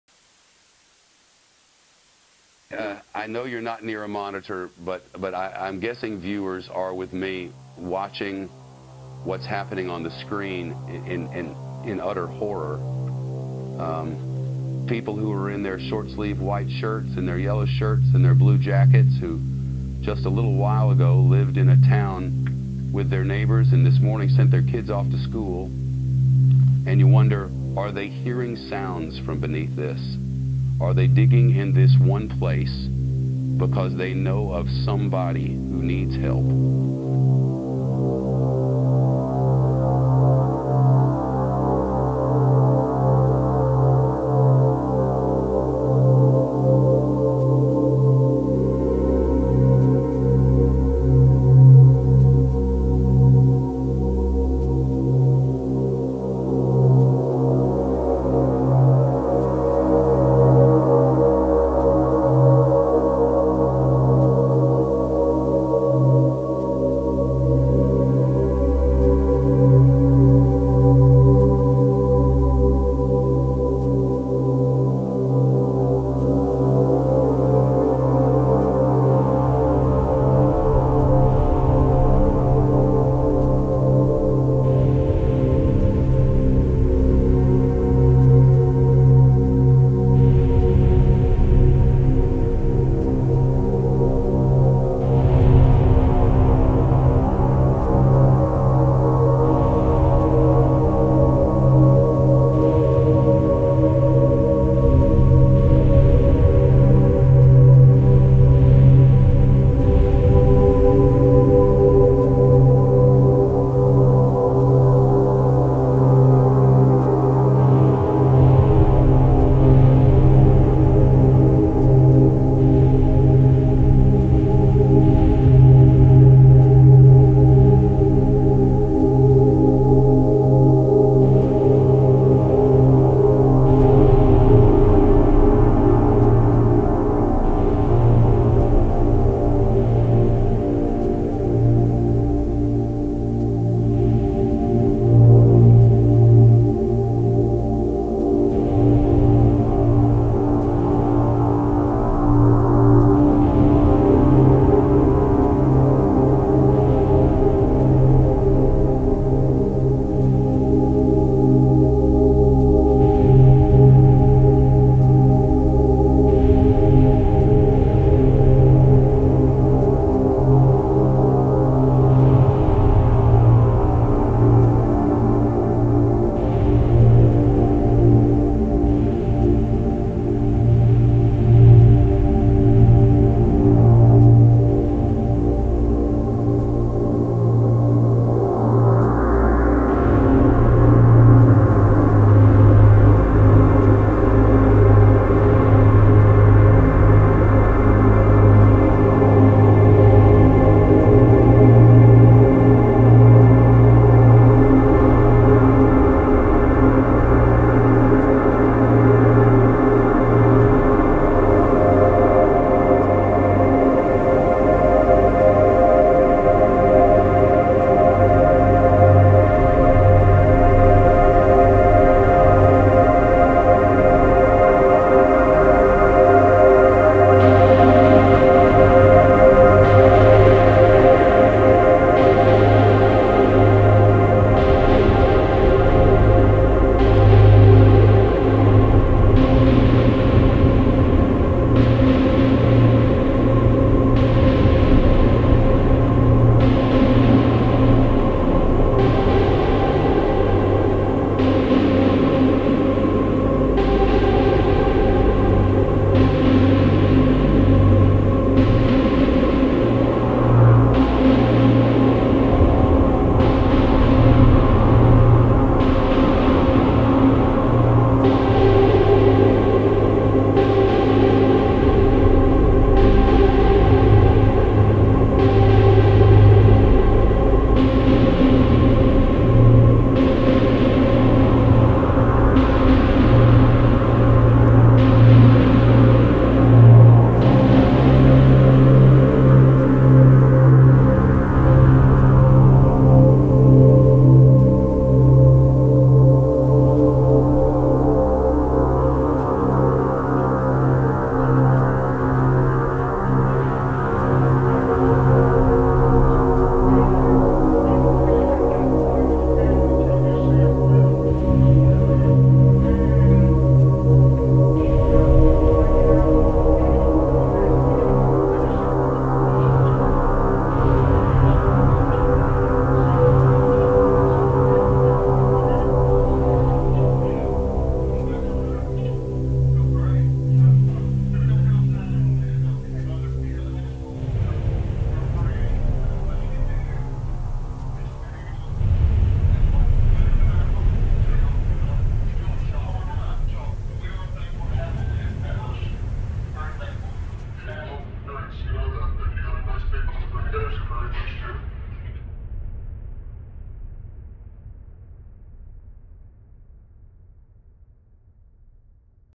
"Beneath the Rubble" - The Oklahoma Tornado Disaster - a film by Panayioti Yannitsos
"Beneath the Rubble" is a collection of images from close to 3 weeks after an E5 tornado struck the city of Moore, Oklahoma on May 20th, 2013.

The tornado killed 23 people and injured an additional 377 others.  An estimated 1,150 homes were destroyed as most areas in the path of the storm suffered catastrophic damage.

This is the story of what was left behind.

Directed and Edited by Panayioti Yannitsos.
Original Score composed by Jamie Spittal.